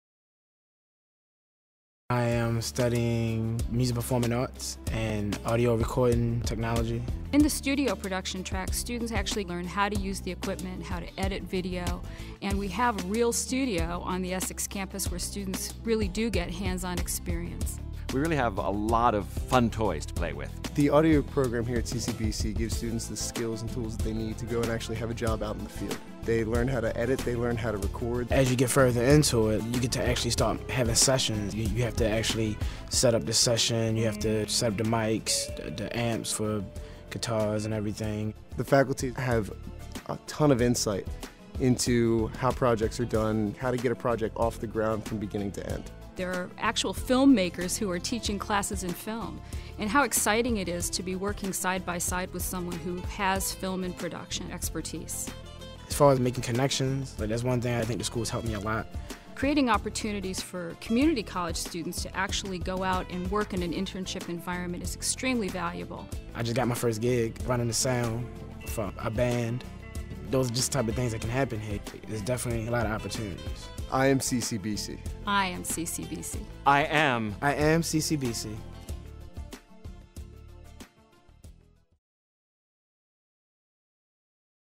Communications and Production
Describes CCBC's programs in Corporate Communication, Studio Production, Music Recording and more!